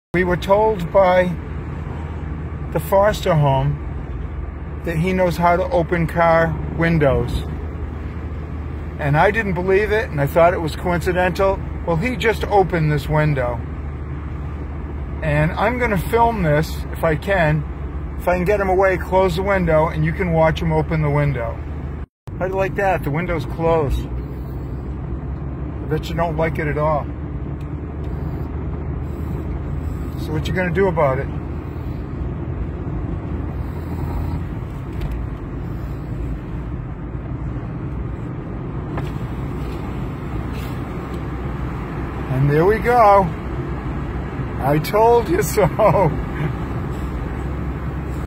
Lhasa Apso can open car windows!
I was warned that the new Lhasa rescue dog could open car windows and I couldn't believe it. until.